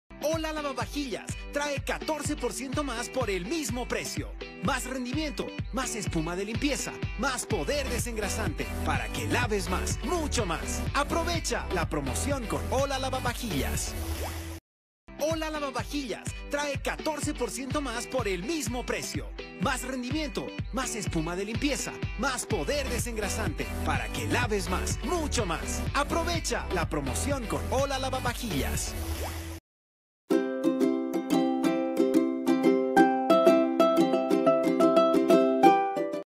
OLA LAVAVAJILLAS - Con 14% mas de contenido (Bolivia 2021)
Comercial de OLA: Ola lavavajillas. Con 14% mas de contenido en el envase.